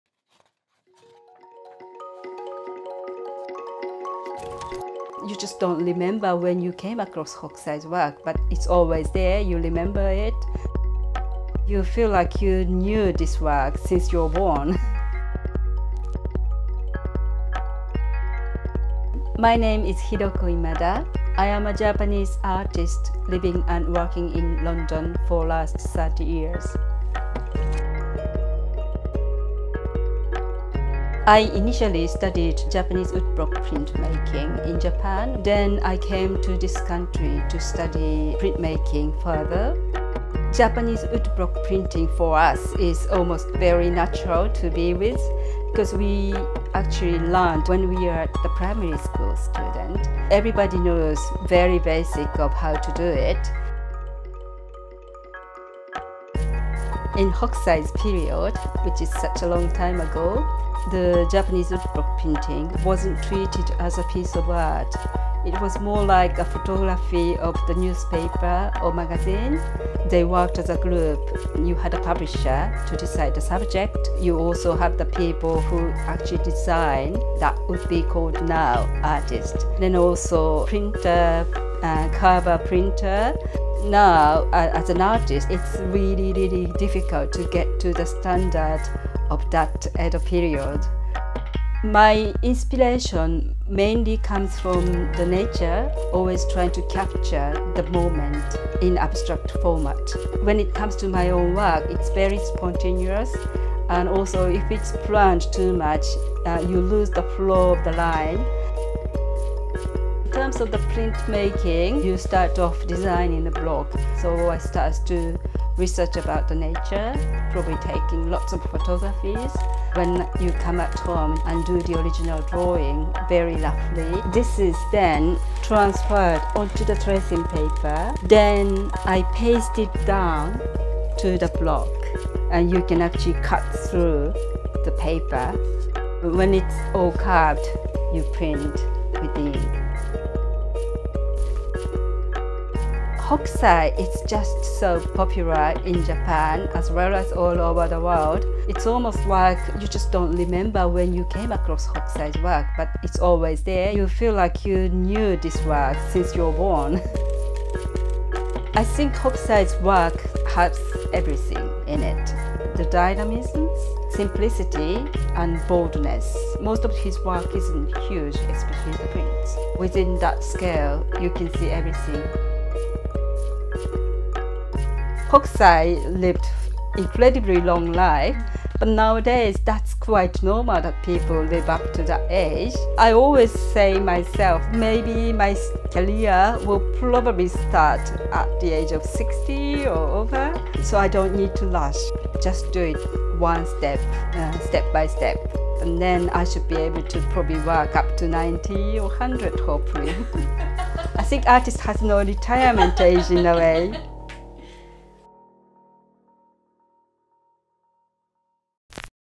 DM's x THE MET | Hiroko Imada
To mark the launch of our collaboration with The Met, we asked artist Hiroko Imada to create a Hokusai-inspired woodblock print commemorating the partnership. We spent the day with Hiroko in her studio to learn more about the enduring art of Japanese woodblock printing.

© The Metropolitan Museum of Art. Your purchase supports The Met's collection, study, conservation, and presentation of 5,000 years of art.